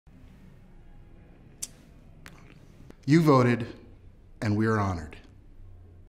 ESPN's 5-Word Speech at the 22nd Annual Webby Awards
Rob King, Senior VP - Original Content, News & Digital Media, accepts the People's Voice Award for Sports in Mobile Sites & Apps.